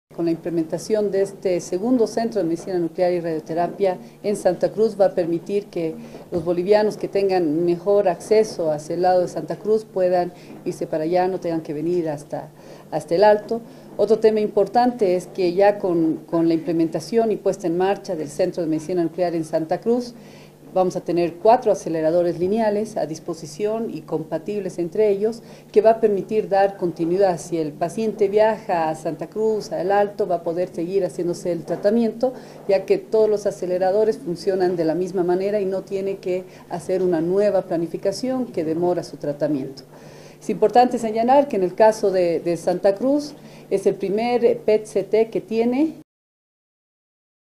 APERTURAN SEGUNDO CENTRO DE MEDICINA NUCLEAR
Centro de Medicina Nuclear en SantaCruz abre sus puertas desde este lunes para pacientes con cáncer.